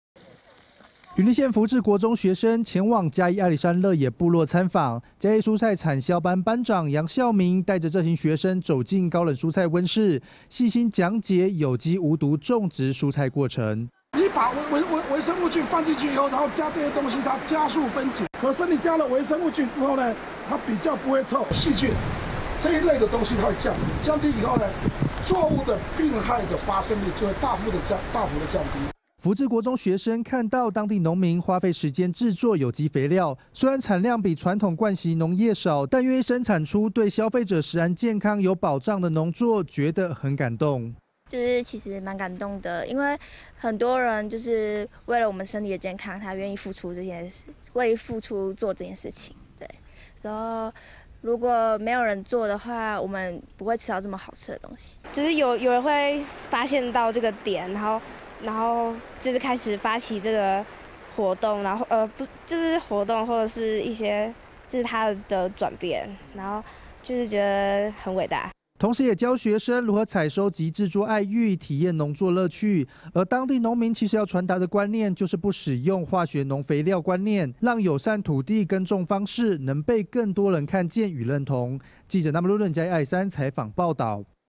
福智國中訪樂野 展開農業文化交流 2017-05-23 TITV 原視新聞
雲林縣福智國中學生，前往嘉義阿里山鄉樂野部落參訪，嘉義蔬菜產銷班班長楊孝明，帶著這群走進高冷蔬菜溫室，細心講解有機無毒種植蔬菜過程。

福智國中學生看到當地農民，花費時間製作有機肥料，雖然產量比傳統慣習農業少，但願意生產出對消費者食安健康有保障的農作，覺得很感動。

同時也教學生如何採收及製作愛玉，體驗農作樂趣。

而當地農民其實要傳達的觀念，就是不使用化學農肥料觀念，讓友善土地耕種方式，能被更多人看見與認同。